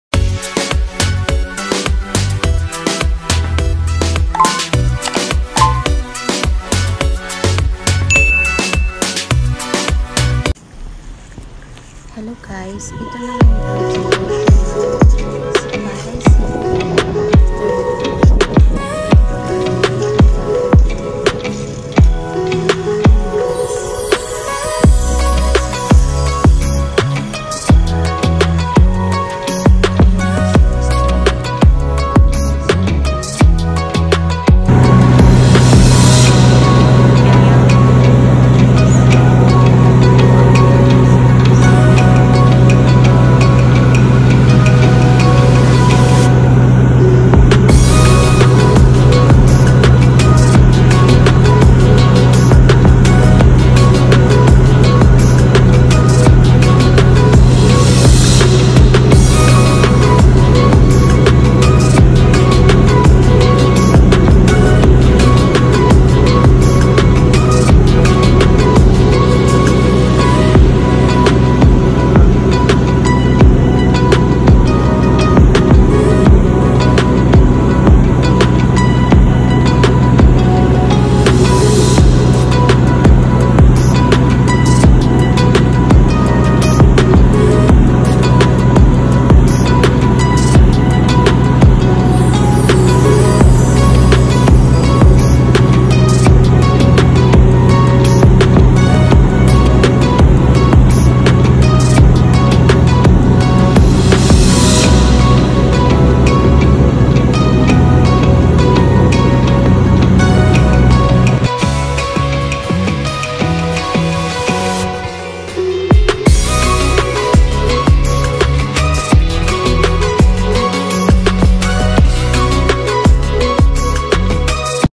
Updates Sipalay City Negros Occidental sitwasyun ngayun after ng bagyong odette +Aerial viewing😢
Sipalay updates after ng bagyong odette.

Sa lahat ng taga Sipalay walng kuryente sira mga poste totally black out po.

Bka months pa at ma ayus at maibalik ang mga ilaw

Laban lng Sipalaynon importante safe

## bagyongodette